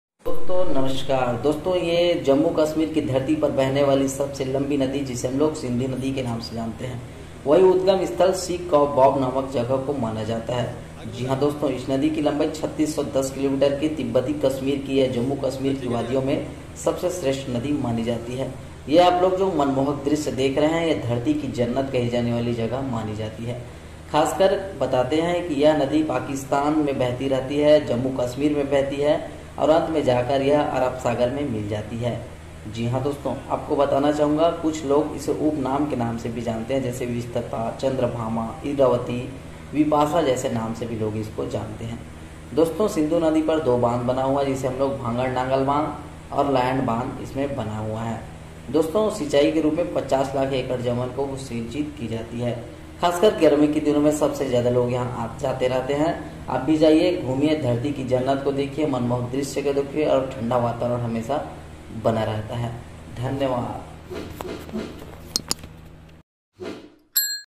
भारत के तीसरी सबसे लंबी नदी long rivers india
एशिया की सबाएलंबी नदी जिसे हम सिंधु नदी के नाम से जानते है जम्मू कश्मीर की तिब्बत में बहुती रहती है जो सबसे बड़ी रूप में जानी जाती है उद्गम स्थल तैब्बत को मानी जाती है सिंग का बाब के नाम से जानती है जो सबसे रूप में भी कही जाती है जिसकी बहुत ही जानकारी आप li ja sakti hai jiski anginat rup me bhi kahi jati hai wahi easkinlanbai ke rup me 3610 kilomitar ki sabse lambi jagah bhi kahi jati hai jiski bahut hi jankari aap log li ja sakti hai jyadatar ansh pakistan me bhi bahti rahti hai jiski anginat ke rup me ki ja sakti hai wahi sindhu nadi jo sabse lambi nadi ke naam se bhi jani jati hai vistata chandra Bhama ilaychi vidahata ke naam se jani jati hai bandh bhagad nagal bandh, layad bandh
bharat ko sabse badi tisri badi jagah bhi kahai jati hai सिंचाई के रूप 50 लाख एकड़ भूमि को सिंचित किया जाता है जो सबसे बड़ी जगह के रूप में भी जानी जाती है जिसकी सबसे बड़ी जानकारी ली जा सकती है इसकी मुख्य रूप से झेलम नदी , चुनाव , रवि , व्यास नदी को मानी जाती है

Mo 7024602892
all india national park rivers temple tour fort animal knowledge